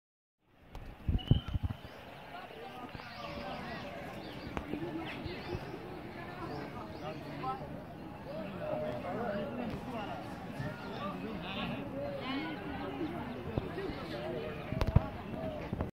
Garden Sounds at the Taj Mahal
I really liked this garden right next to the Taj Mahal.